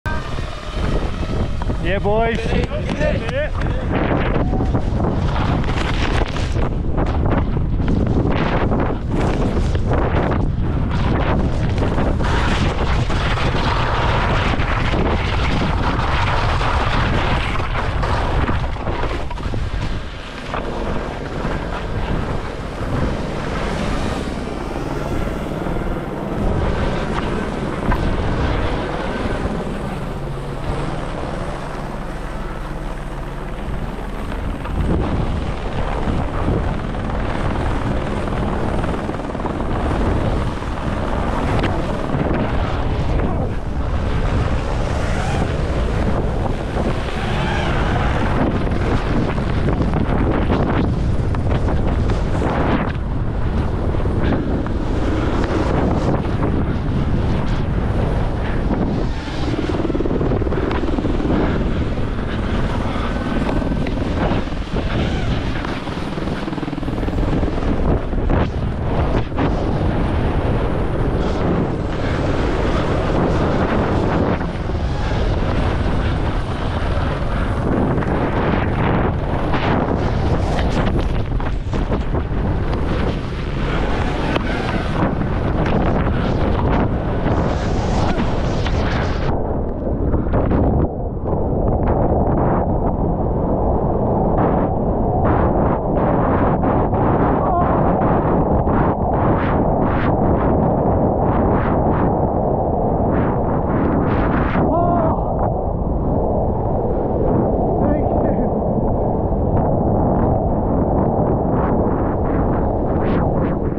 GoPro Drone FPV POV: Finn Bilous' World First 360 off the Hollywood Cliff I 2024 YETI Xtreme Verbier
Finn Bilous (NZL) made history in Verbier, being the first rider ever to 360 off the Hollywood Cliff, which helped him on his way to third.